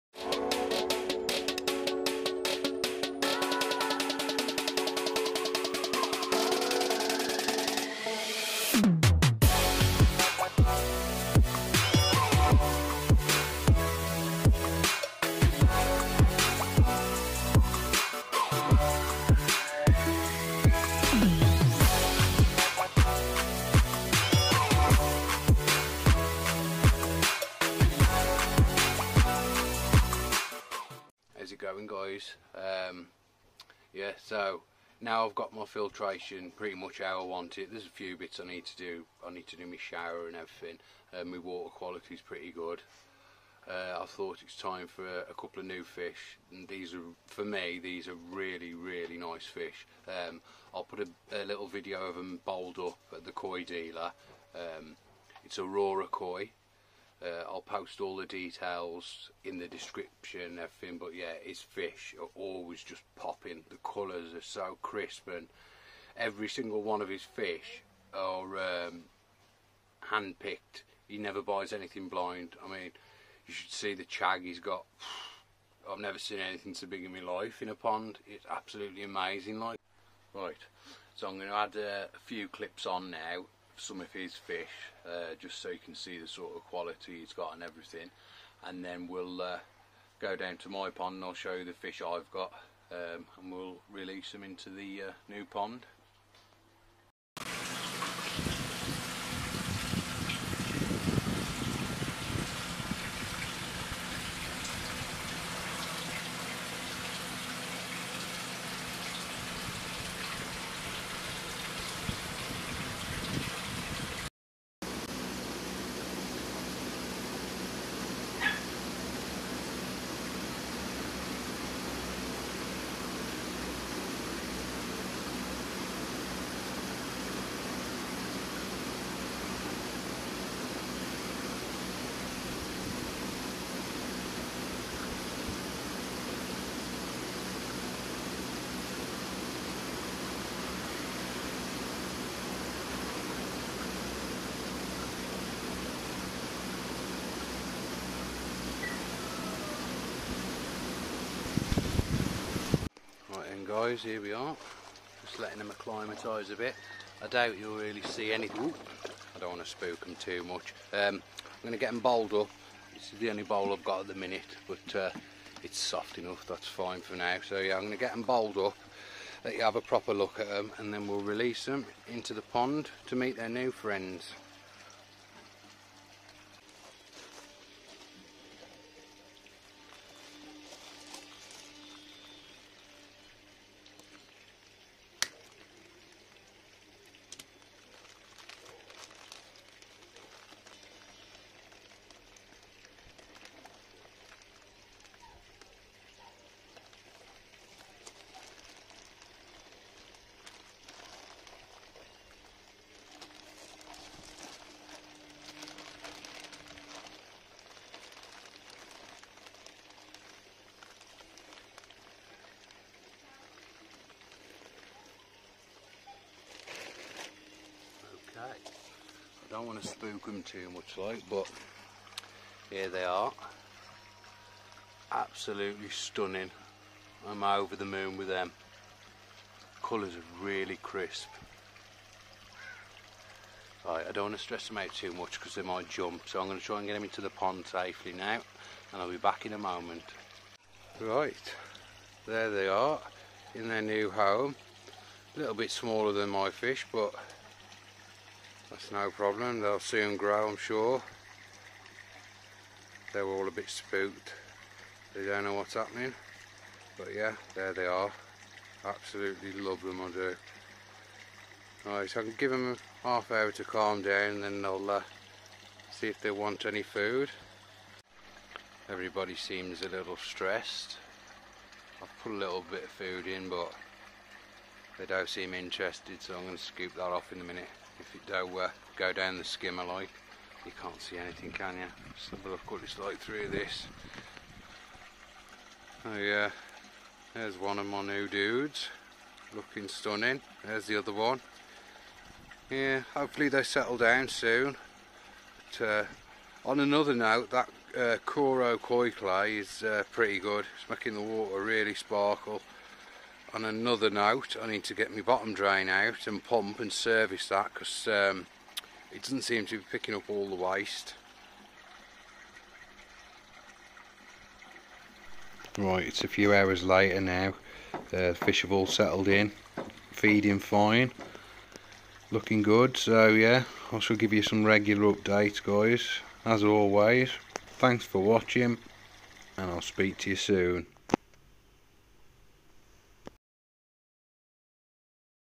Adding new koi to my pond
In this video I will be adding two new koi to my pond.

I show you a small selection of the stunning Japanese koi available at Aurora koi

You can find Aurora koi on Facebook or at the website above.